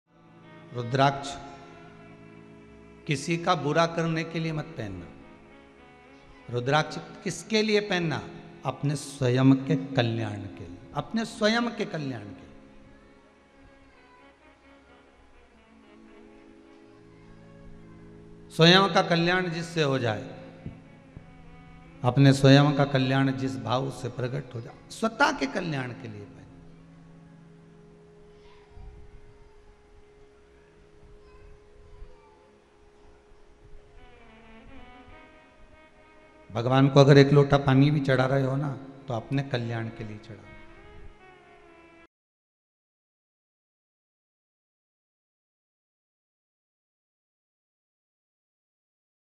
जानिए रुद्राक्ष धारण करने के लाभ।। Pujya Pt. Pradeep Mishra Ji ।। Aastha Channel
जानिए रुद्राक्ष धारण करने के लाभ

10 वें BCS रत्न अवॉर्ड 2019 में आस्था चैनल बना नंबर 1 आध्यात्मिक चैनल।

4- शनिवार को पीपल के नीचे दिया जलाने से क्या होता है ।। Pardeep Mishra Ji ।। Aastha Channel